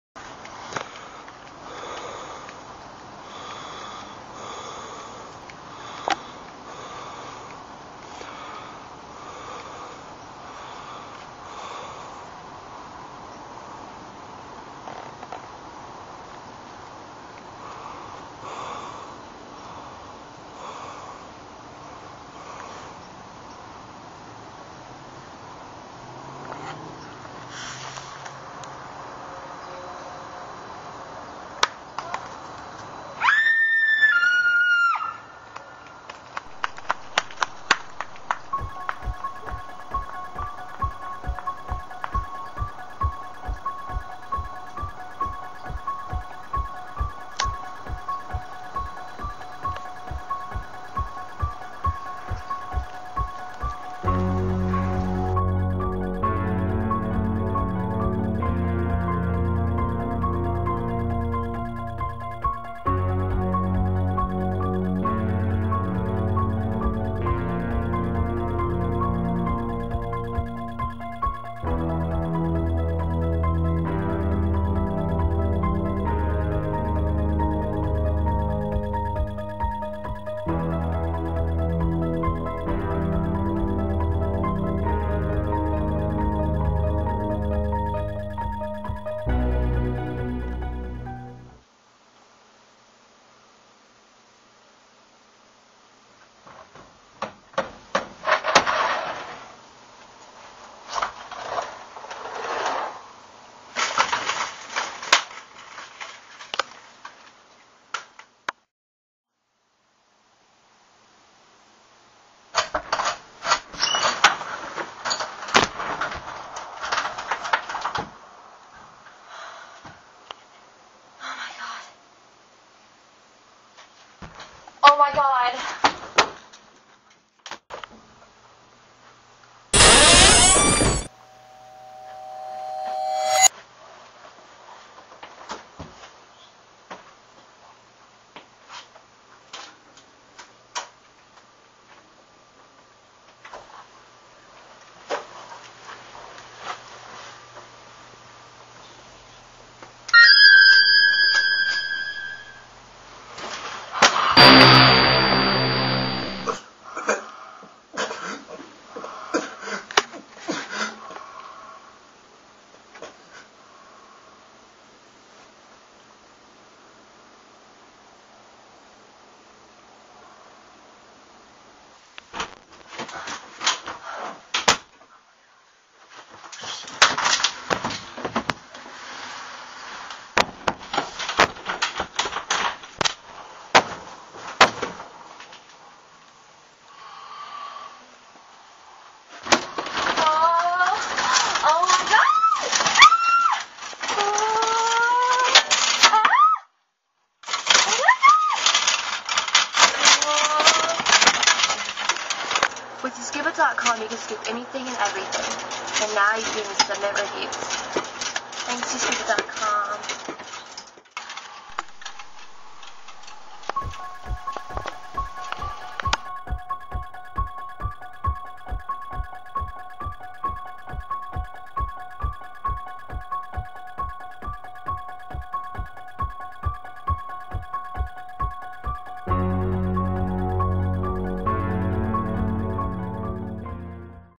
Another Halloween Movie
Michael Myers returns to take the lives of Sissy and Trip...